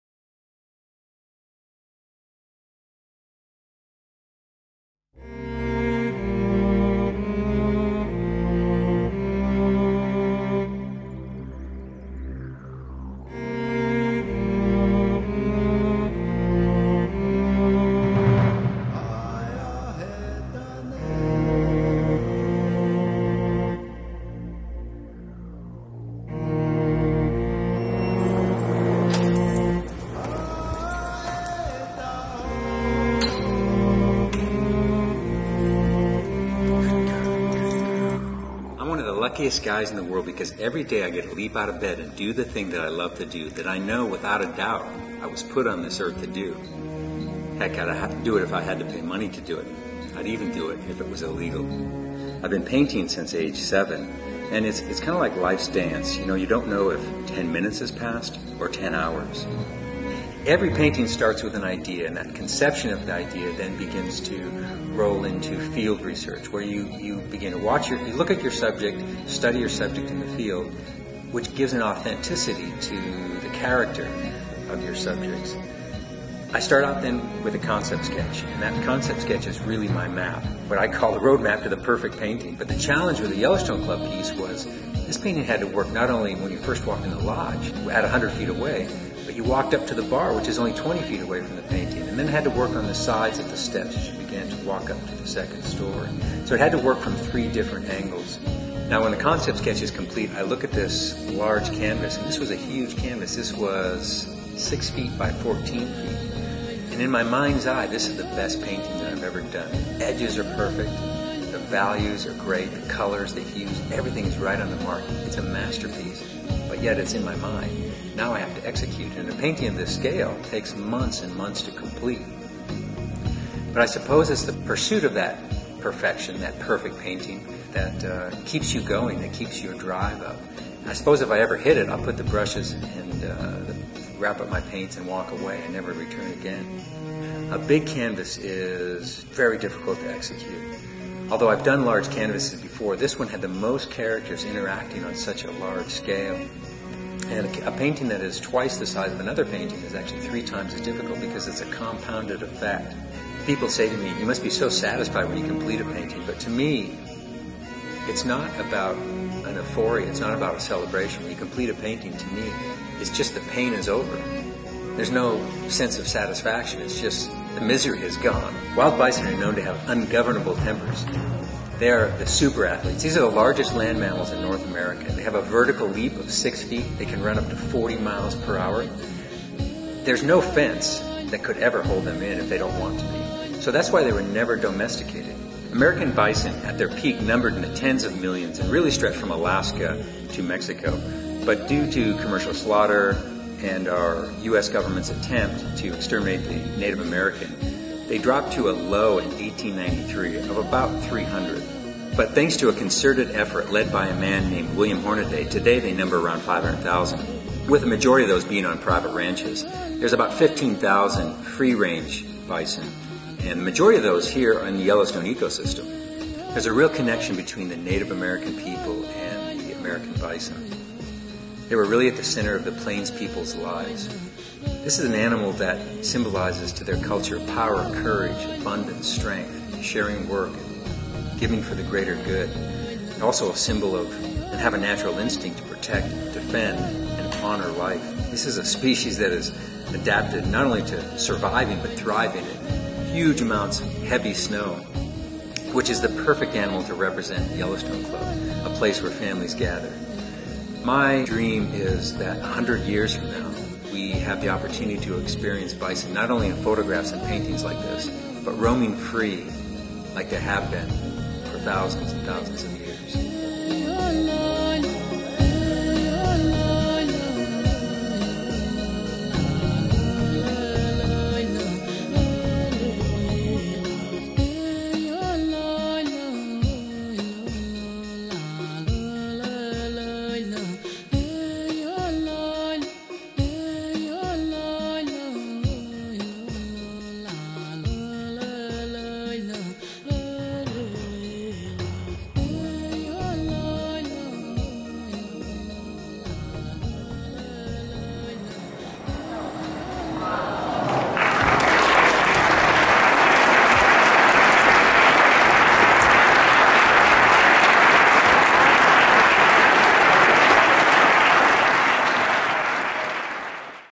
"Cold Air - Deep Powder" - Banovich Commission for the Yellowstone Club
On December 27th, 2012 we unveiled "Cold Air-Deep Powder" at the Yellowstone Club in Big Sky, MT. This detailed painting of a herd of bison was commissioned by the club's Warren Miller Lodge. The vibrant and action-packed scene features the animals stampeding through deep snow with a massive framed size of 8' x 16', making it one of John Banovich's largest paintings. "It's perfect," said Sam Byrne, whose firm Crossharbor Capital owns the private resort, "I have always had a vision of what a Banovich painting would look like in this space and he executed it perfectly. We wanted to create something experiential for the Yellowstone Club, and he nailed it."
Upon viewing the painting it is easy to feel the cold wind and imagine the sound of thundering hooves, churning through the billowing snow. The herd of bison features a family unit (mature bulls, cows, calves and young adults), reflecting the Yellowstone Club`s mission as a place where families gather. The snow flies as the herd surges through deep fresh powder. The ice and snow on fur combined with the animals` warm breath puffing from their lungs resonates with those that have had the privilege of cutting through fresh morning powder.

"I wanted the painting to show the familial aspect of the Yellowstone Club with bison in all stages of life. And of course you have the snow, which is an integral part of the club and animals native to this region, plowing their way through amazing powder. In my work as a conservationist I have found so many situations where wildlife is beleaguered by humans but this shows a native animal in a natural setting unfettered by humans and civilization. One hundred years from now I hope we are blessed to live in a world where this iconic beast roams free." -John Banovich